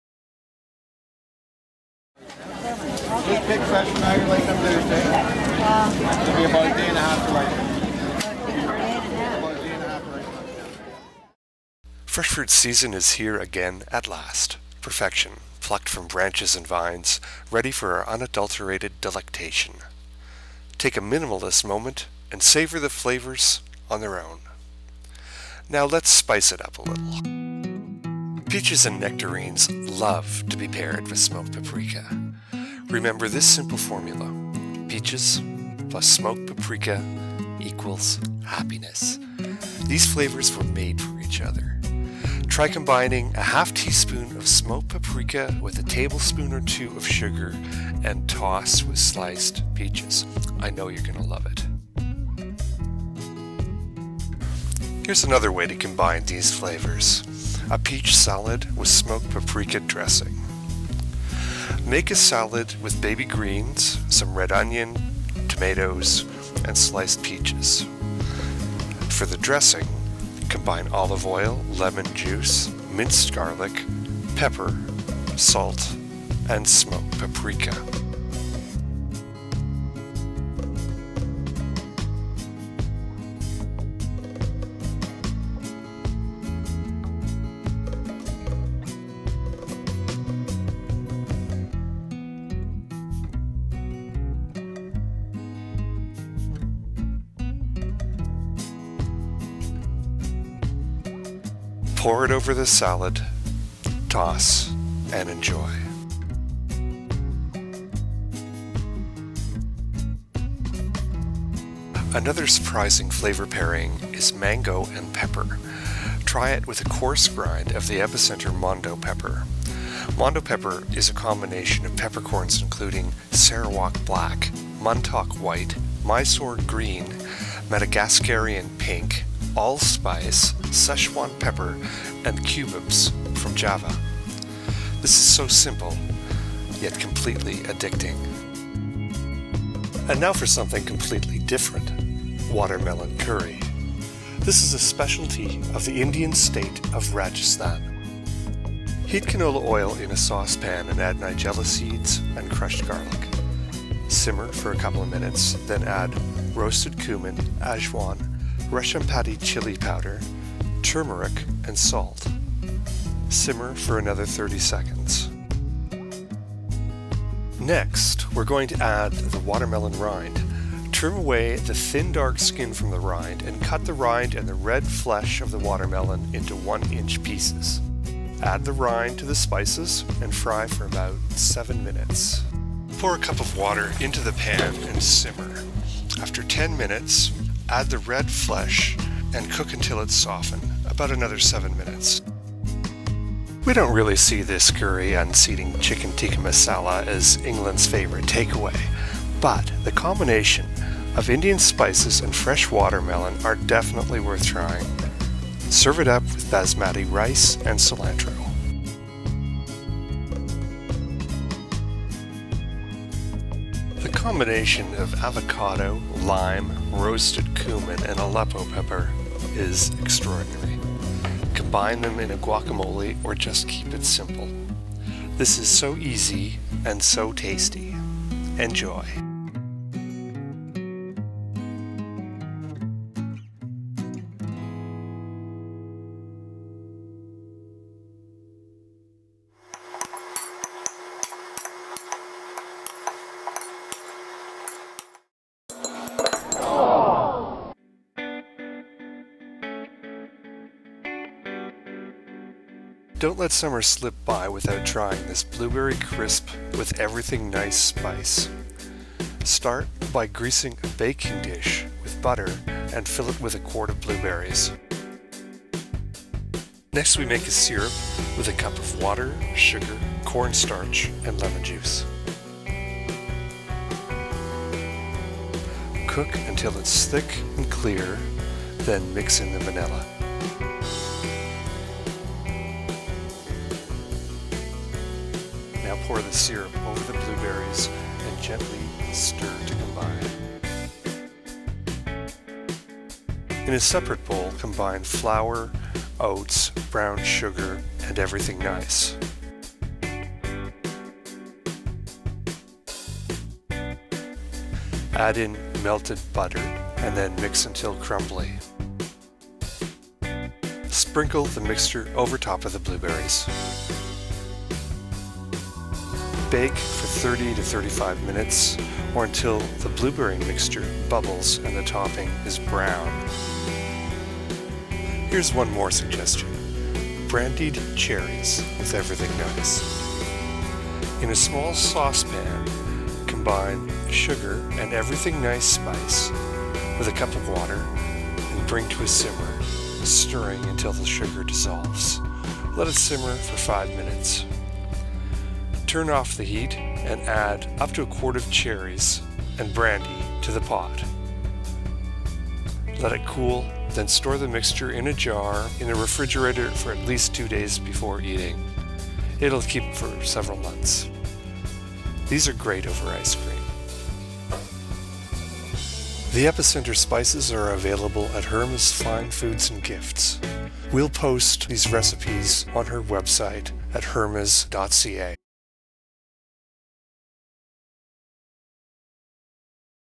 Spicing Summer Fruit with Epicentre Spices
Herma’s presents some unusual recipes for pairing spices with summer fruit. Try Mondo pepper with mango or roasted cumin and Aleppo pepper with lime and avocado. Learn how to combine peaches with smoked paprika. The video includes an excellent recipe for blueberry crisp using Everything Nice baking spice. Brandied cherries are also nicely spiced. There is also a wonderful recipe for watermelon curry using Indian spices.